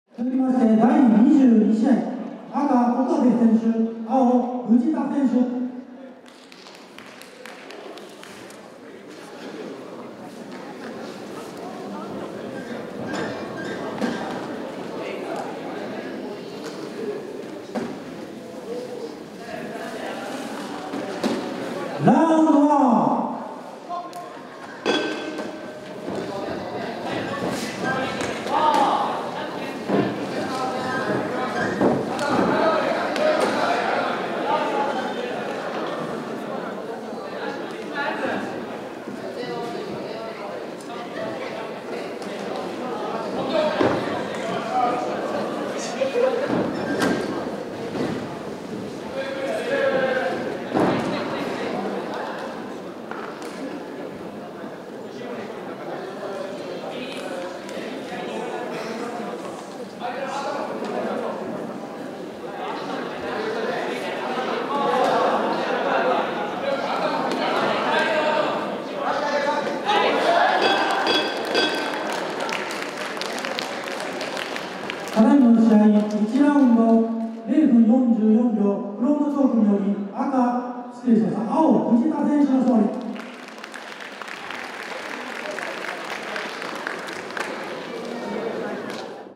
岡部 怜央 vs 藤田 太陽【MMA西日本アマチュア大会】
MMA西日本アマチュア大会
2023年10月22日

【スタンダードクラスルール】
第22試合
70.3kg以下級
岡部 怜央 (STAKE GYM) vs 藤田 太陽 (パンクラス大阪)